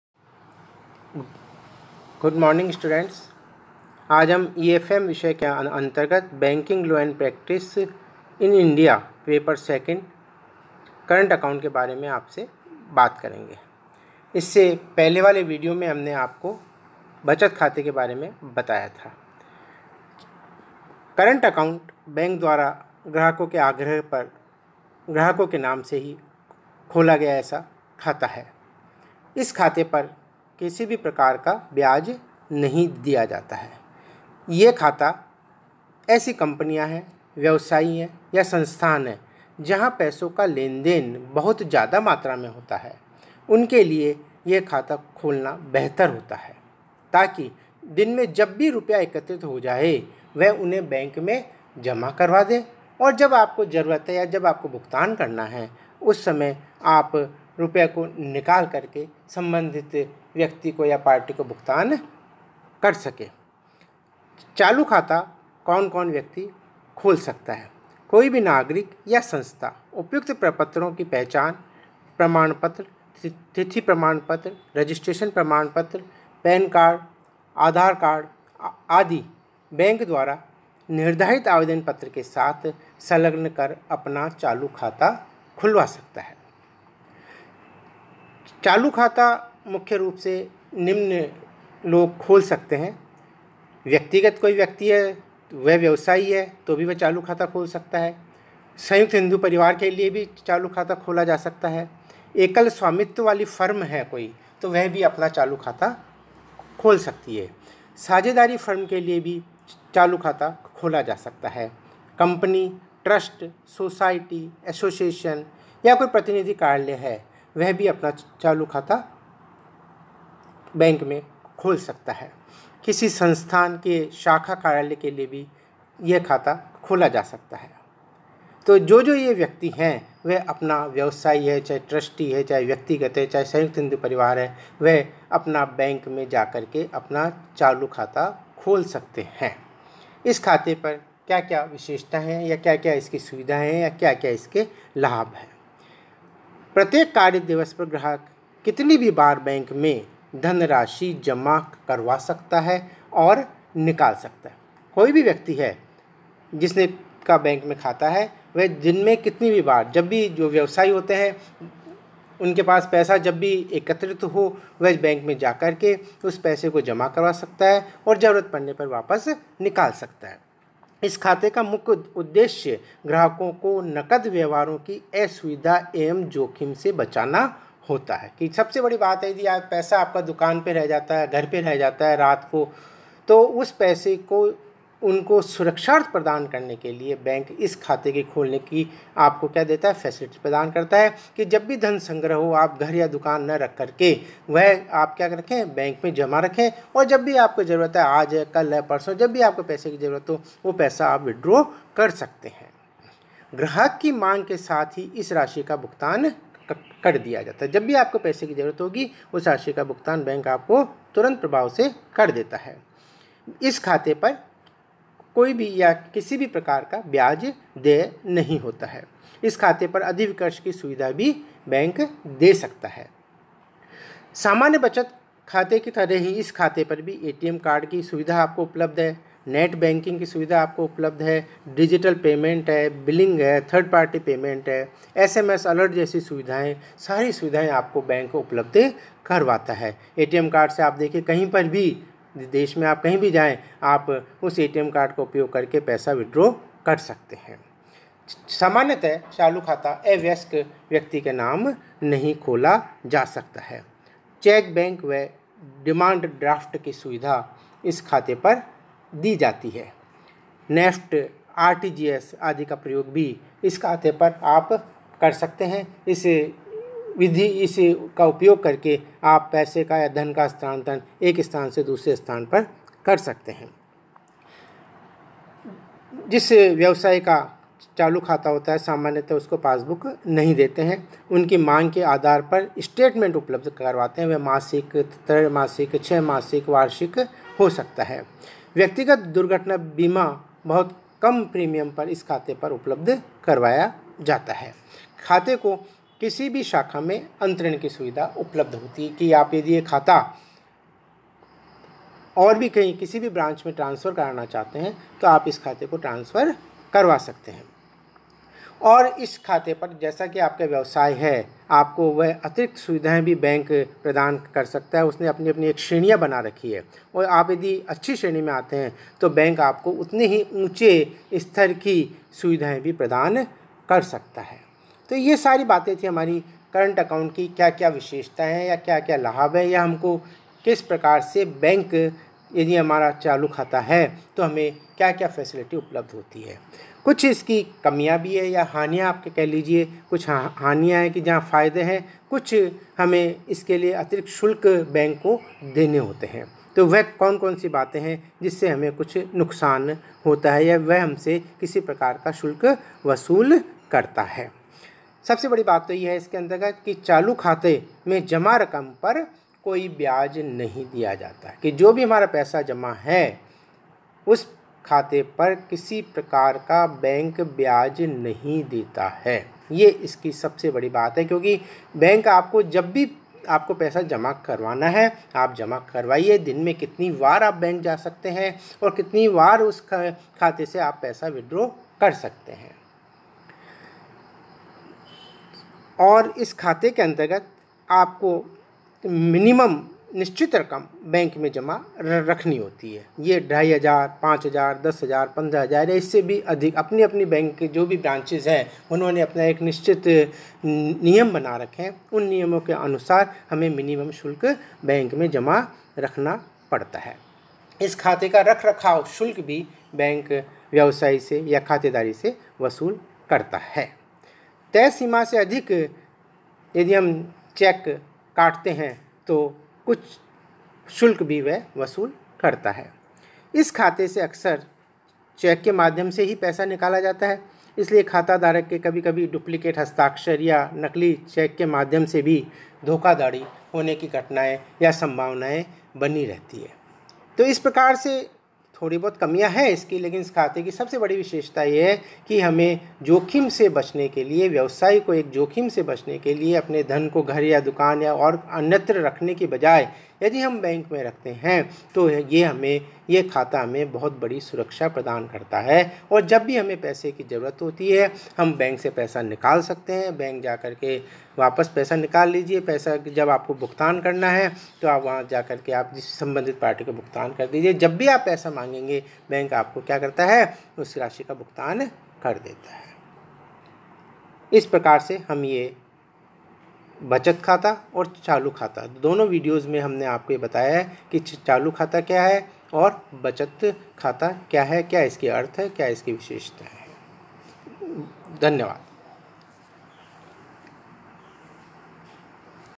SRKP...M.K.Ranka
B.Com Part II EAFM II Current Account by M.K.Ranka , Associate Professor SRKP Govt College